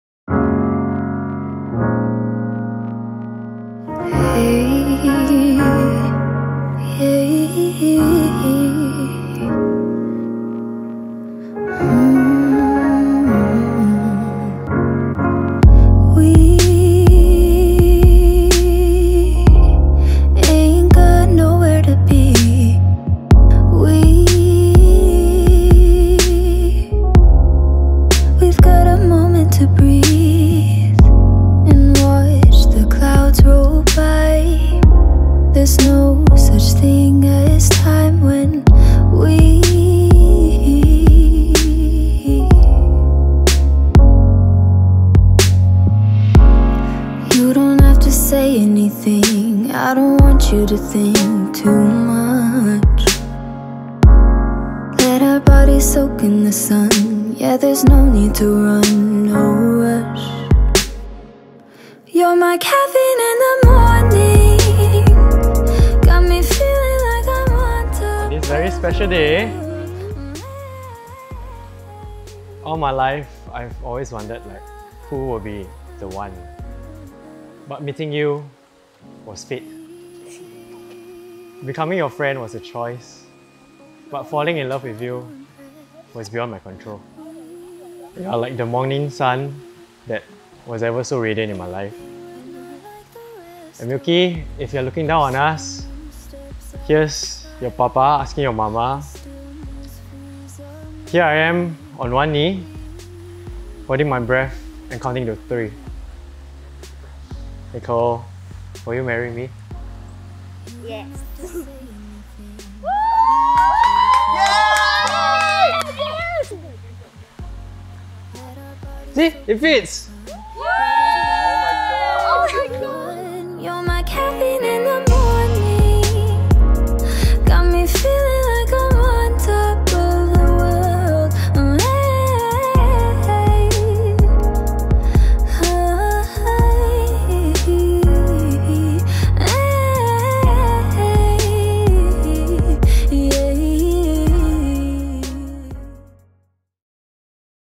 The Proposal 💍 | James Seah and Nicole Chang Min
Guys, I have a fiancé now - HOW COOL IS THAT??!! 😍

I would to hear how we can work together and let our creative juices flow!